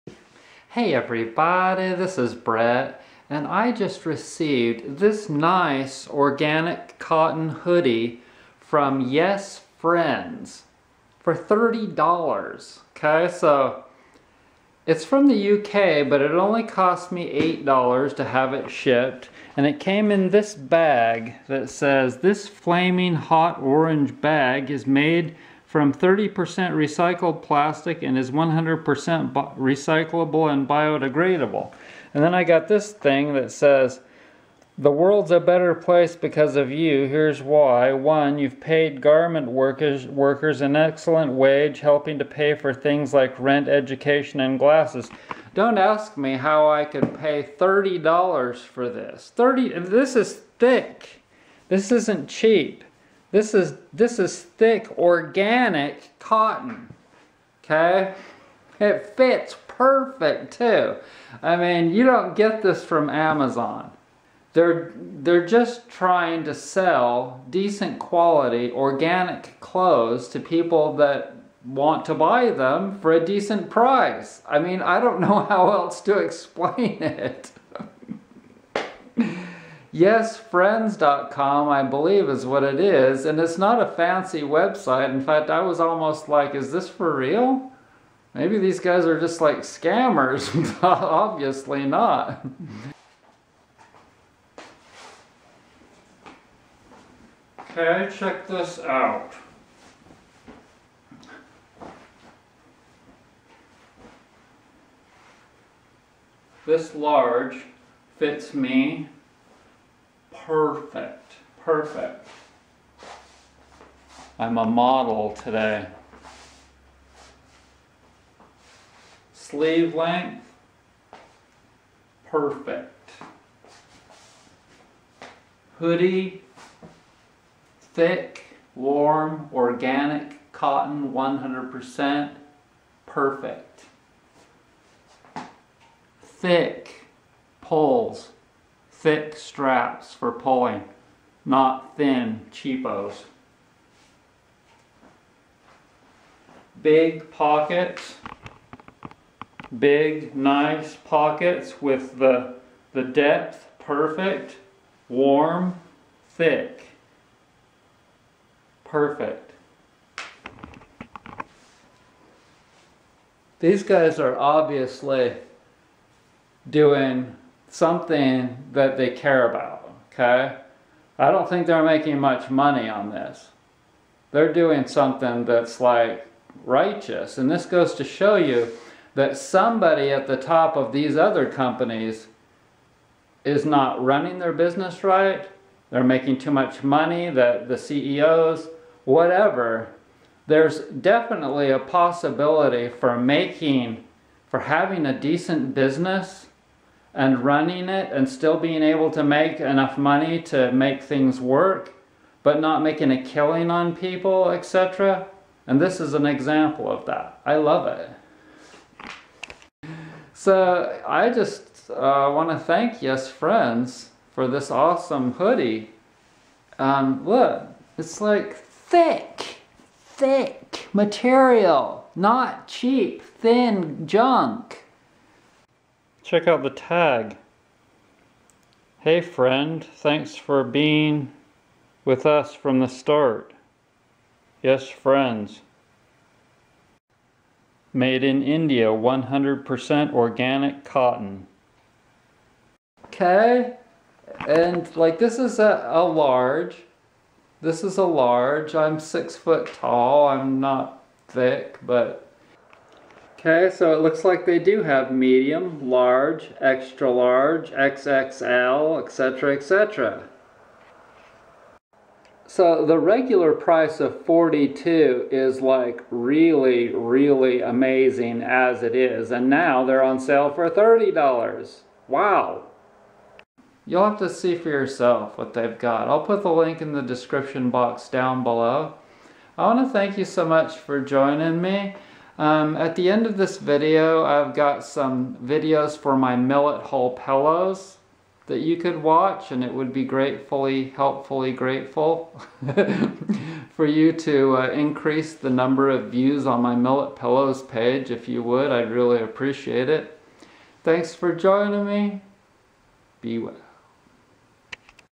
100% Organic Cotton Hoodie Pullover $30!
I bought this 100% organic cotton hoodie for just $30 from Yes Friends!